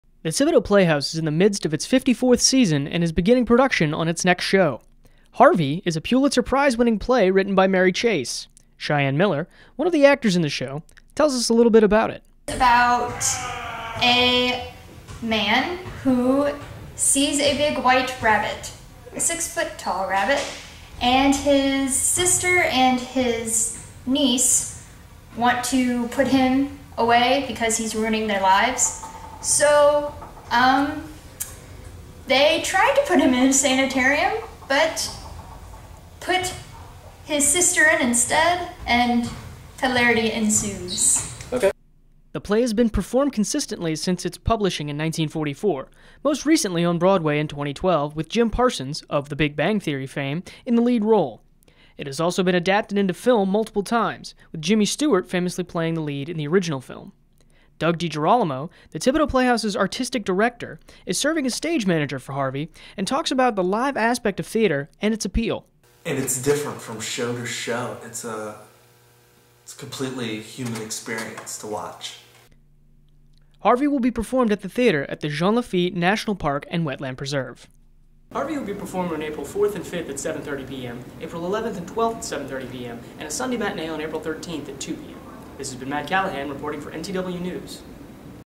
Harvey News Package
"Harvey" is going to be performed by the Thibodaux Playhouse.  Matt Callahan has more.